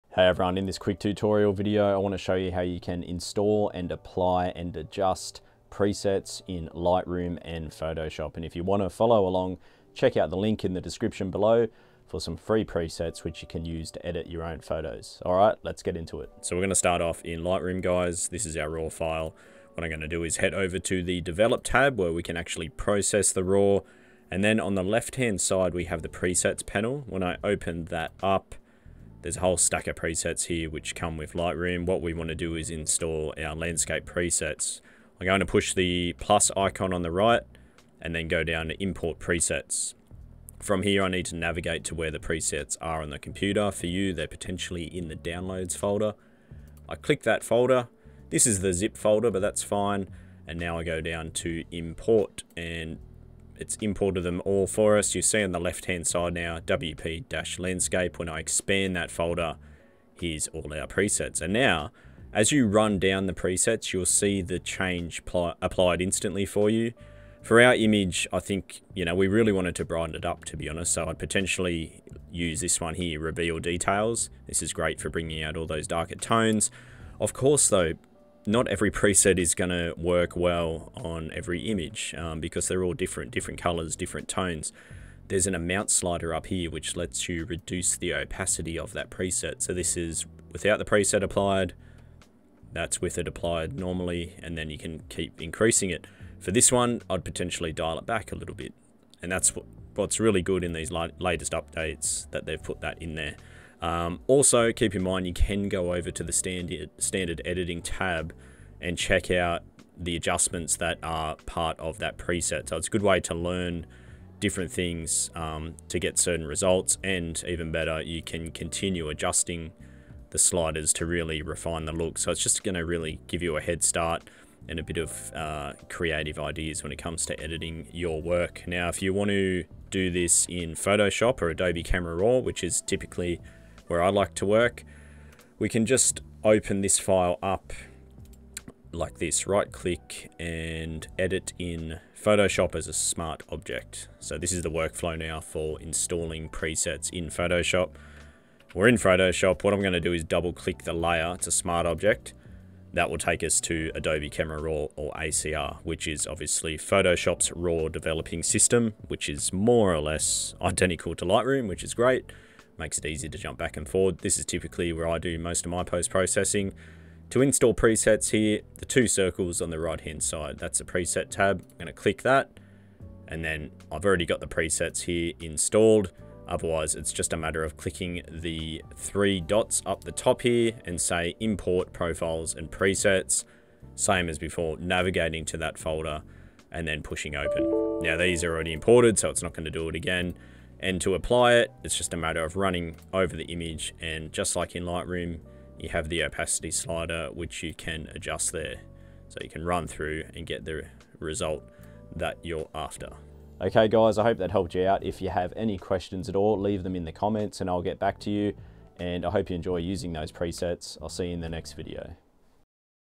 How to Install and Use Presets in Lightroom and Photoshop 2022
In this video, I’ll show you how to install, apply, and adjust presets in Lightroom & Photoshop 2022.

Adobe have made a few changes recently so I wanted to do a quick walkthrough for you!

Using presets is a great way to test different looks and get a quick edit of your images in seconds.

00:00:01 - Intro - Don’t Forget to Grab Your Free Presets 😎
00:00:16 - How to Install Presets in Lightroom
00:01:01 - How to Apply a Preset in Lightroom
00:01:19 - How to Adjust a Preset in Lightroom
00:02:28 - How to Install a Preset in Photoshop / Adobe Camera RAW (ACR)
00:03:10 - How to Apply a Preset in ACR

Say hello!